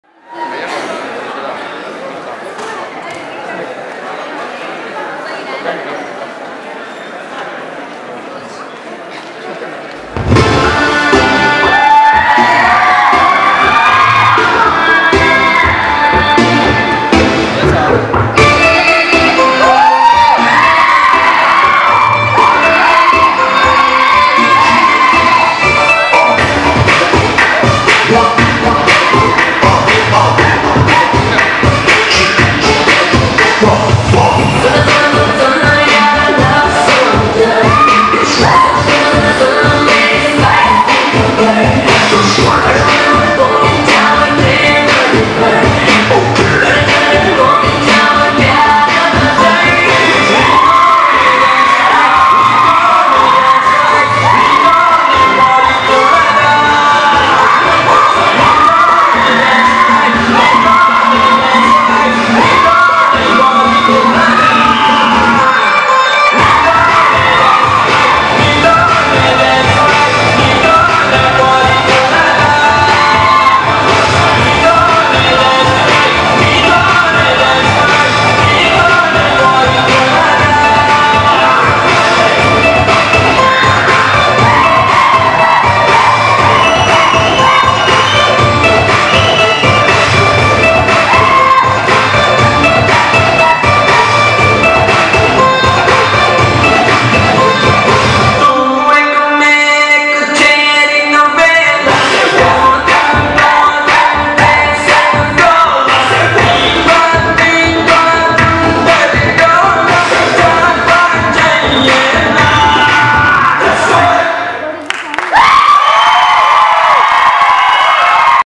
Neethone Dance to Night Dance by TCA IIT KGP students
Dancers: Daya Kiran, Vamsi Vinay, Shashank, Hruthik, Uday Bhaskar, Nikhil, Ravi Teja